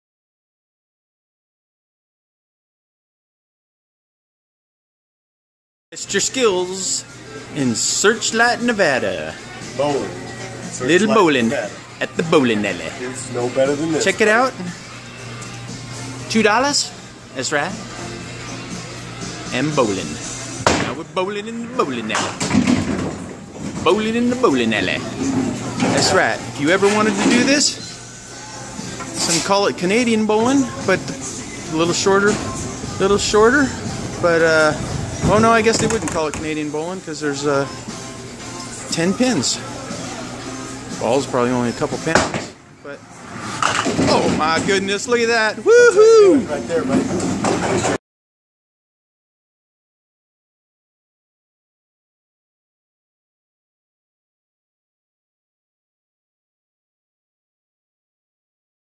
Bowling at Searchlight
Mini Bowling In Searchlight Nevada -Terribles Casino Sept. 2015